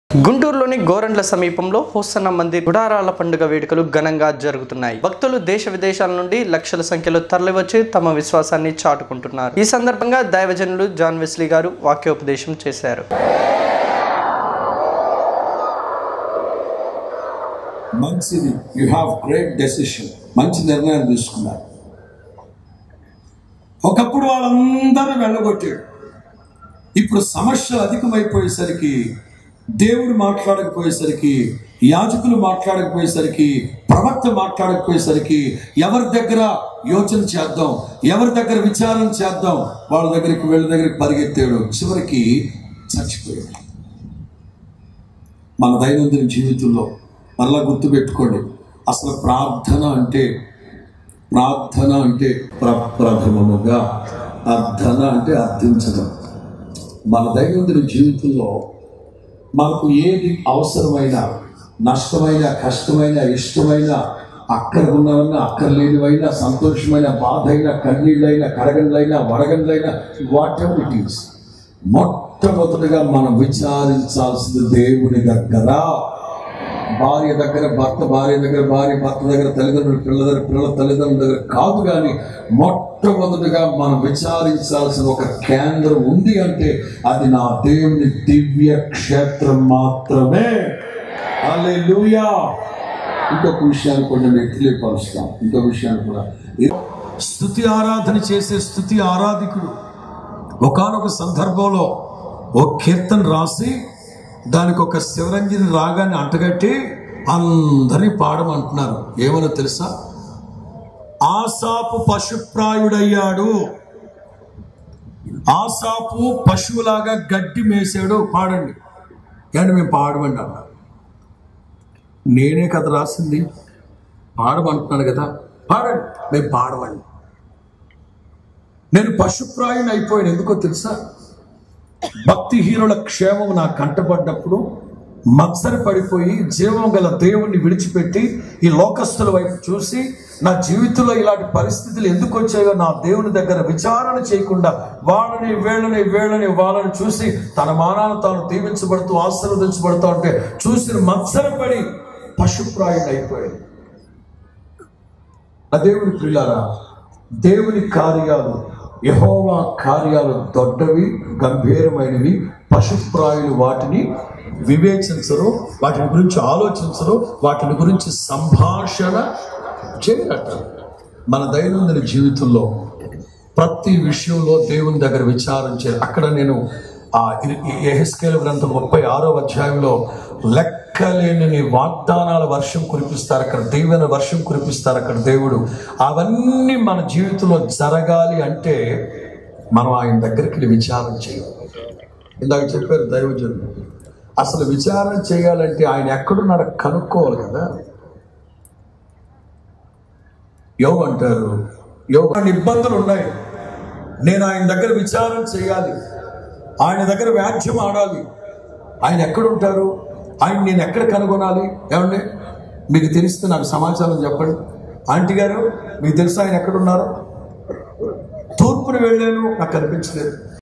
గోరంట్ల సమీపములో హోసన్నా మందిర్ గుడారాల పండుగ వేడుకలు
గుంటూరు లోని గోరంట్ల సమీపములో హోసన్నా మందిర్ గుడారాల పండుగ ఘనంగా జరుగుతుంది. భక్తులు దేశ విదేశాల నుండి లక్షల సంఖ్యలో తరలివచ్చి తమ విశ్వాసాన్ని చాటు కొన్నారు. ఈ సందర్బంగా దైవజనులు వెస్లీ గారు వాక్యోపదేశం చేశారు.